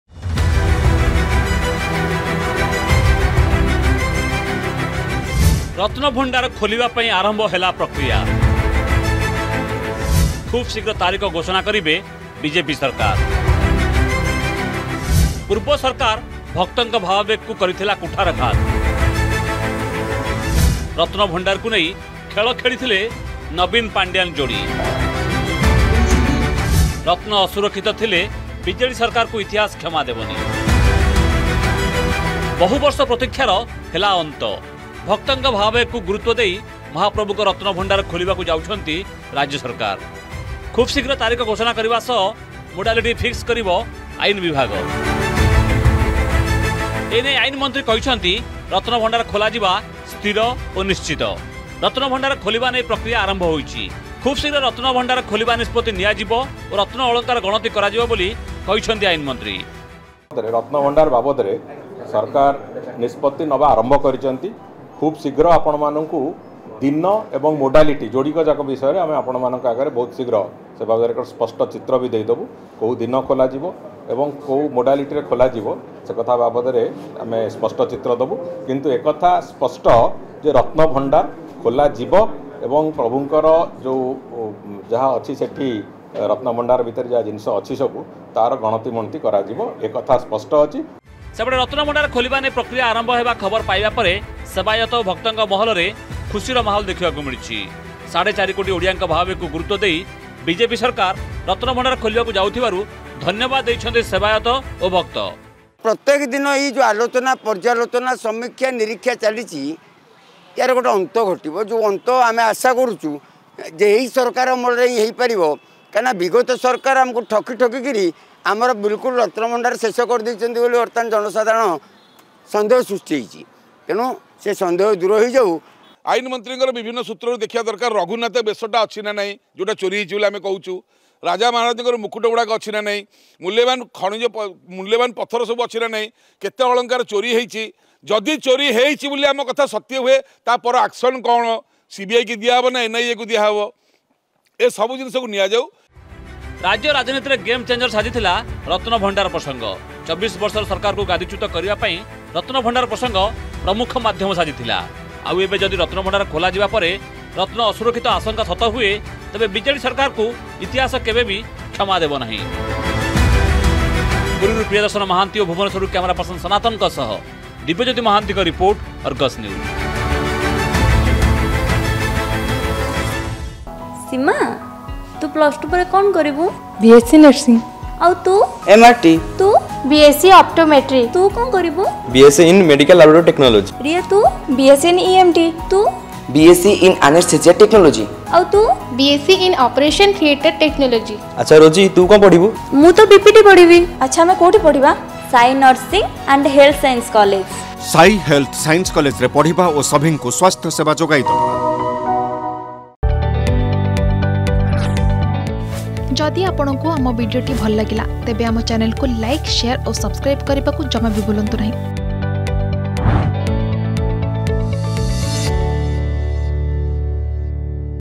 ରତ୍ନଭଣ୍ଡାର ଖୋଲିବା ପାଇଁ ଆରମ୍ଭ ହେଲା ପ୍ରକ୍ରିୟା || Ratnabhandar Will Be Opened || Puri
ରତ୍ନଭଣ୍ଡାର ଖୋଲିବା ପାଇଁ ଆରମ୍ଭ ହେଲା ପ୍ରକ୍ରିୟା । ଖୁବଶୀଘ୍ର ତାରିଖ ଘୋଷଣା କରିବେ ବିଜେପି ସରକାର । ପୂର୍ବ ସରକାର ଭକ୍ତଙ୍କ ଭାବାବେଗକୁ କରିଥିଲା କୁଠାରଘାତ । ରତ୍ନଭଣ୍ଡାରକୁ ନେଇ ଖେଳ ଖେଳିଥିଲେ ନବୀନ ପାଣ୍ଡିଆନ୍ ଯୋଡି। ରତ୍ନ ଅସୁରକ୍ଷିତ ଥିଲେ ବିଜେଡି ସରକାରକୁ ଇତିହାସ କ୍ଷମା ଦେବନି ।